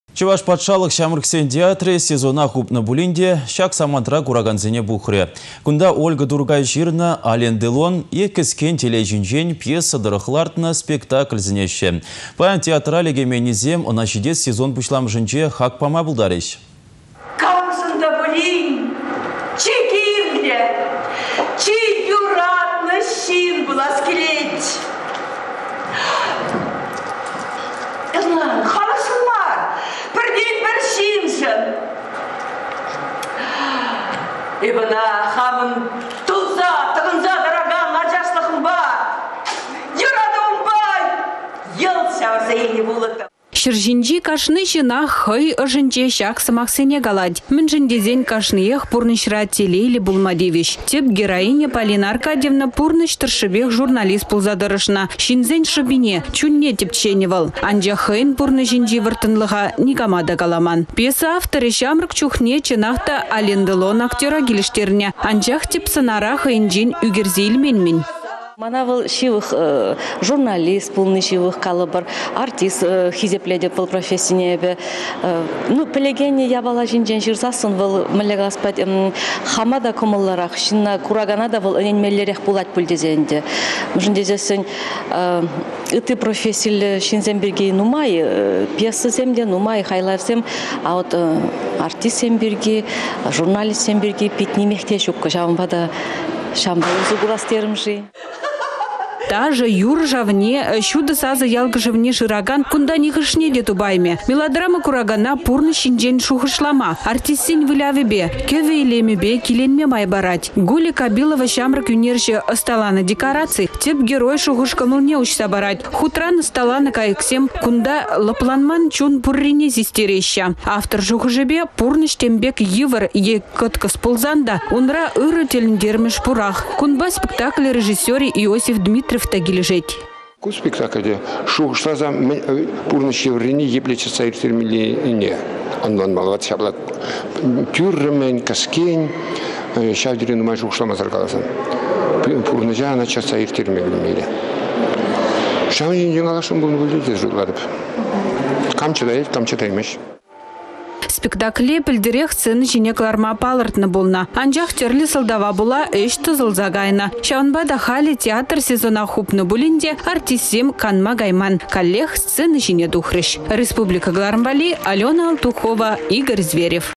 «Ален Делон» чĕнет куракана
Чăваш патшалăх çамрăксен театрĕ сезона хупнă пулин те çак самантра куракансене пухрĕ. Кунта Ольга Туркай çырнă «Ален Делон е кĕскен телей çинчен» пьеса тăрăх лартнă спектакль сĕнеççĕ. Паян театра лекейменнисем ăна çитес сезон пуçламăшĕнче хак пама пултарĕç.